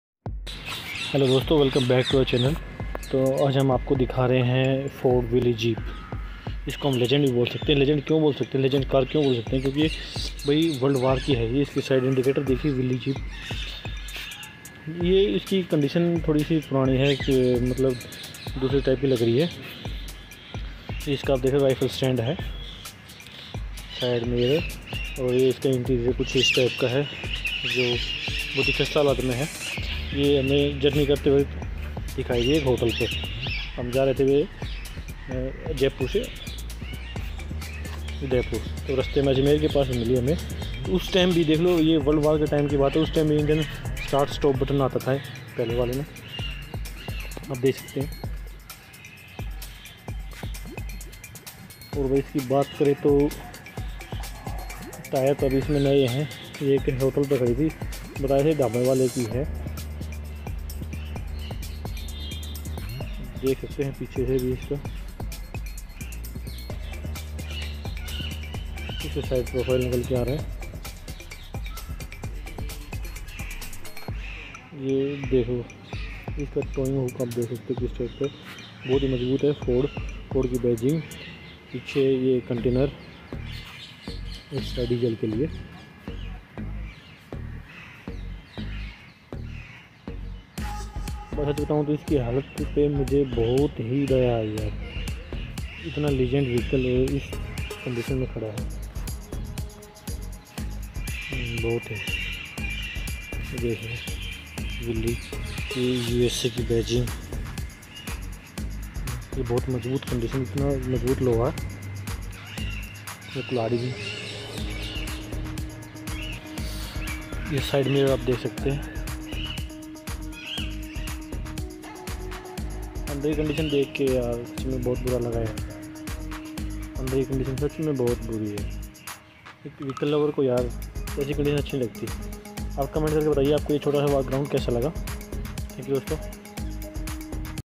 ये क्या कंडीशन कर दी rare व्हीकल ford willyjeep omg 😱😱bad owner.@PRADEEPSIWATCH
Hello friends
welcome to the new video.
aaj ki video hai ford willyjeep ki jo ki hai second world war ki.
for mor information please contact